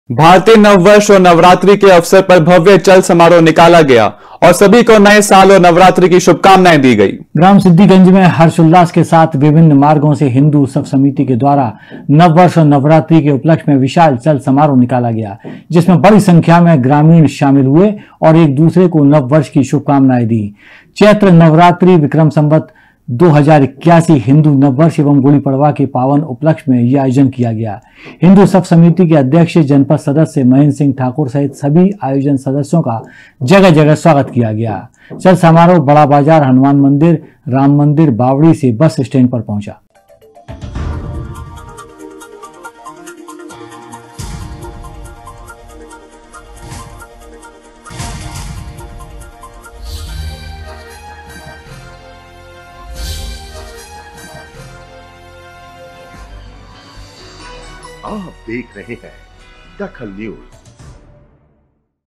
DAKHAL NEWS नव वर्ष और नवरात्रि चल समारोह निकला
नव वर्ष और नवरात्रि चल समारोह निकला
भारतीय नए साल  का मनाया गया जश्न